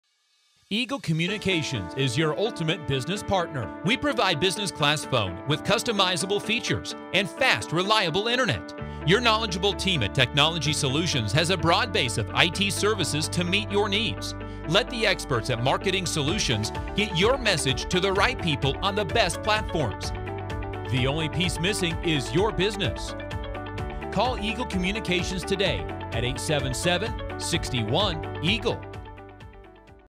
Eagle Communications is your Ultimate Business Partner
Eagle Communications is your ultimate business partner. We provide business-class phone with customizable features, and fast, reliable internet.  Our knowledgeable team at Technology Solutions has a broad-base of IT services to meet your needs. Let the experts at Marketing Solutions get your message to the right people on the best platforms. The only piece missing is your business. to see what we can do for you!